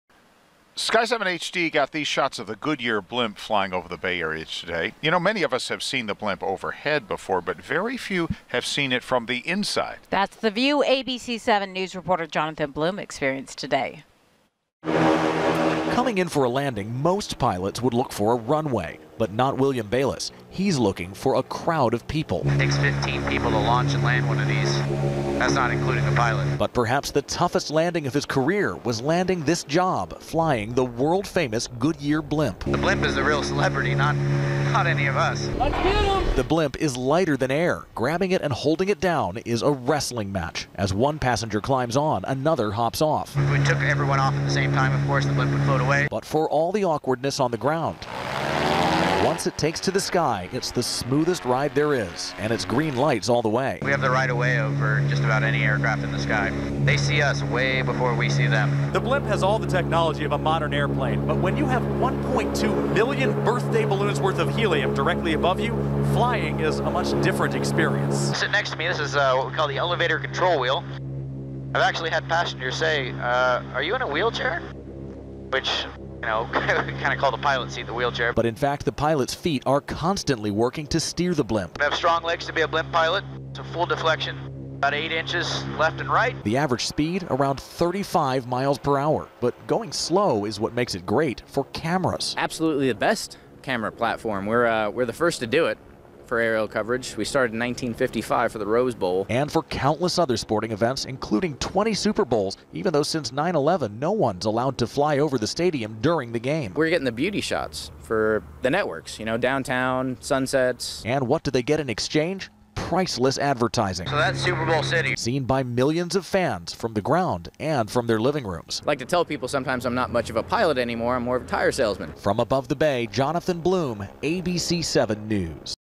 Goodyear blimp pilot flies over Bay Area for Super Bowl
Pilot William Bayliss gave ABC7 News an up-close look at what it takes to fly the world-famous Goodyear Blimp.  Full story